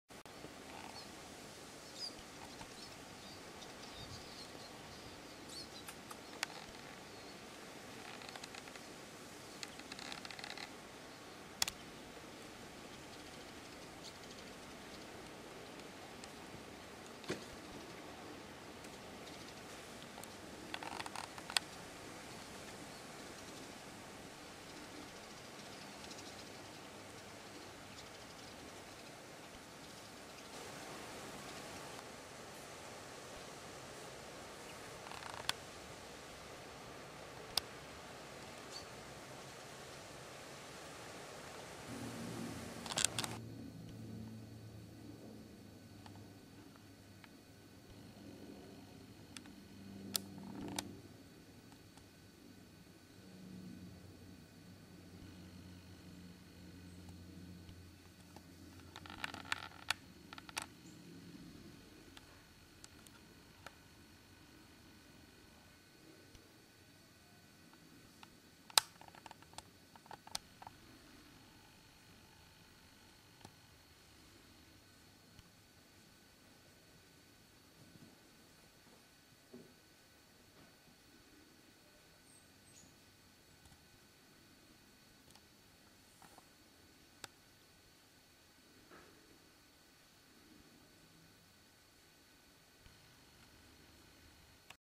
Red squirrel...what a tail!
A very colourful tail to this red squirrel. hence the nickname 'Bandit'